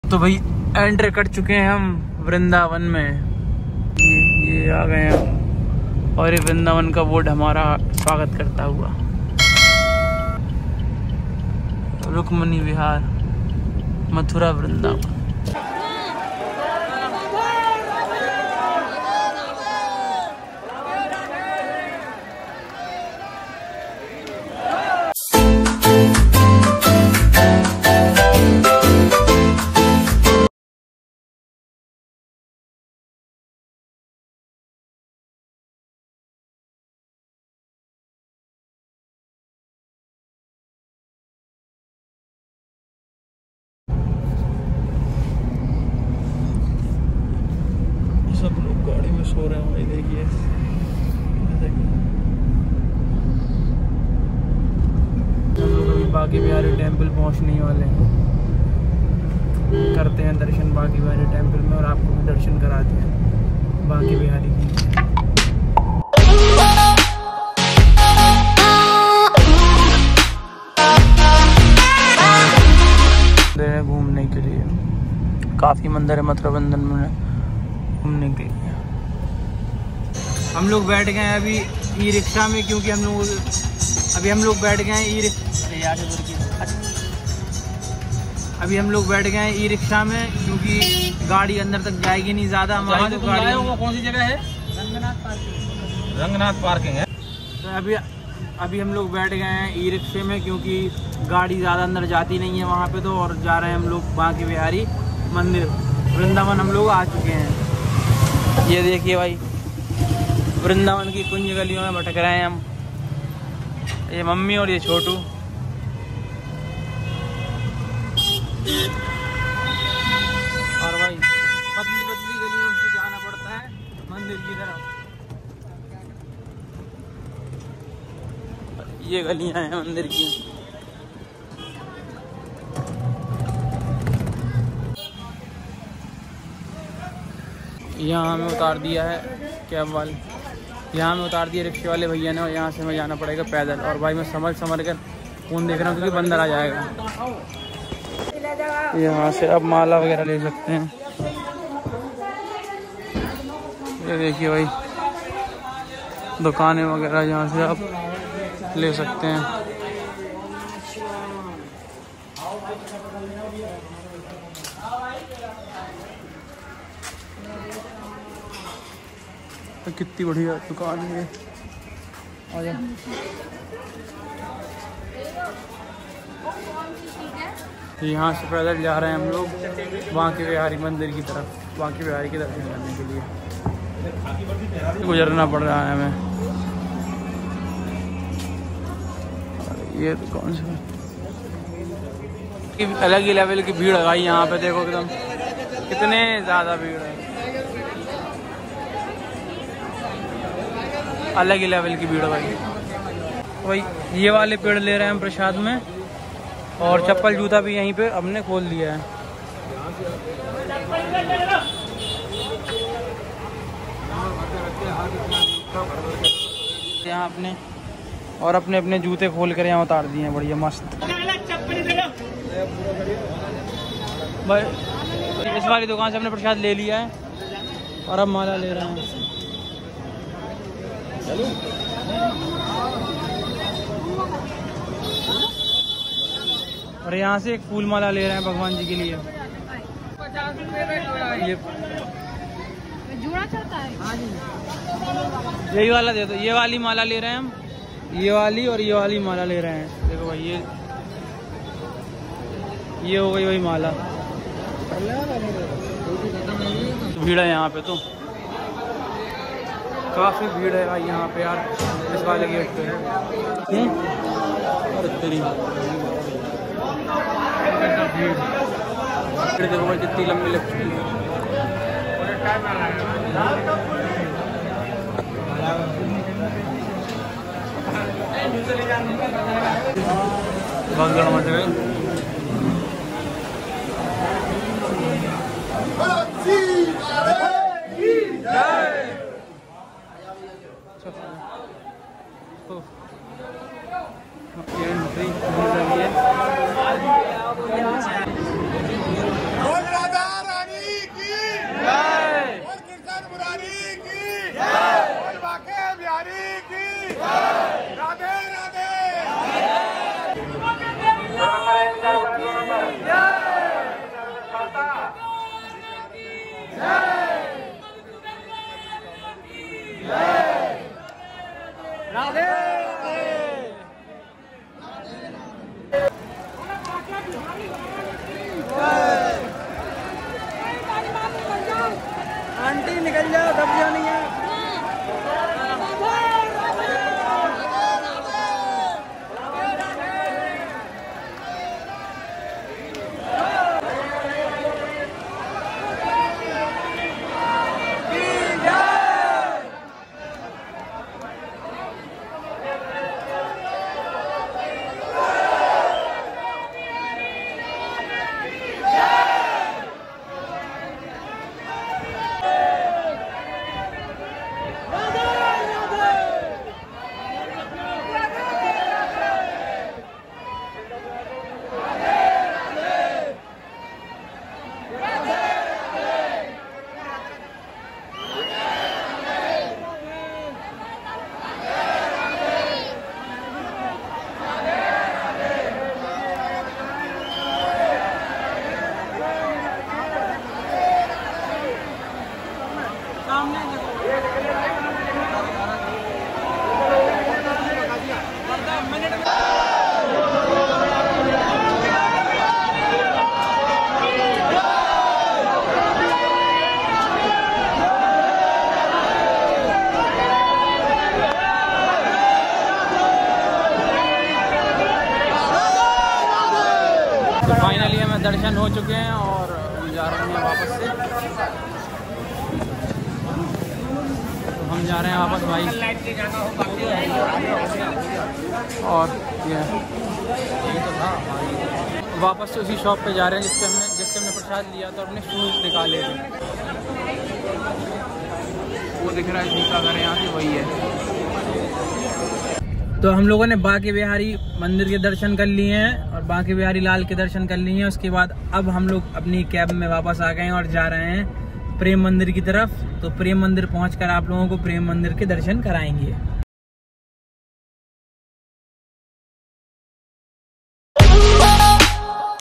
बाँके बिहारी जी ❤️के अद्भुद दर्शन | Banke Bihari Temple Vrindavan Full Tour
श्री बाँकेबिहारी जी के दर्शन , वृंदावन | Magical Darshan of Shri Bankebihari ji Temple | Vrindavan

Hello guys, my name is Prashant and you watching the absolute PS.

बाँकेबिहारी मंदिर, वृंदावन, में स्थित एक प्रमुख हिन्दू तीर्थक्षेत्र है जो भगवान कृष्ण को समर्पित है। इस मंदिर का नाम “बाँकेबिहारी” भगवान कृष्ण के एक प्रिय नाम से आया है। यहां की मूर्ति अद्वितीय है और उसकी आकर्षक मुस्कान देवों के साथ अद्वितीयता का भावना पैदा करती है। मंदिर का निर्माण श्री हरिदास जी द्वारा 1863 में हुआ था और यह विशेष रूप से भक्तों के बीच प्रिय है, जो यहां भगवान के साथ सांग और नृत्य का आनंद लेते हैं।

In this blog, we enjoy a lot.